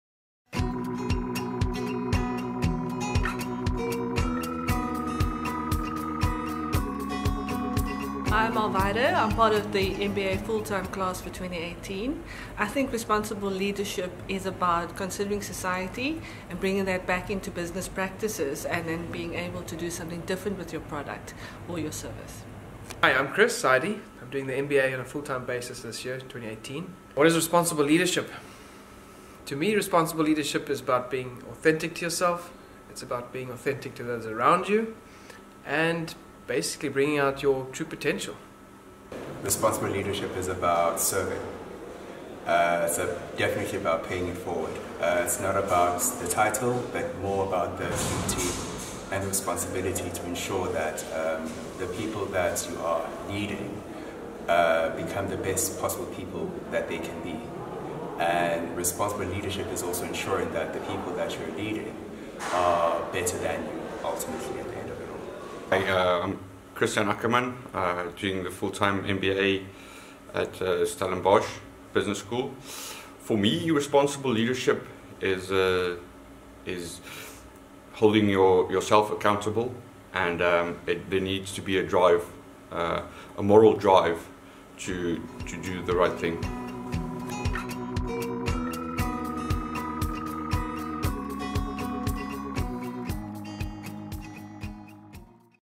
USB Students Have Their Say: What is Responsible Leadership?
At USB we recognise the need to develop responsible leaders, as well as create new knowledge that contributes to better business in better societies all over the world. We asked some of our current students to give their definition of responsible leadership. Watch it now!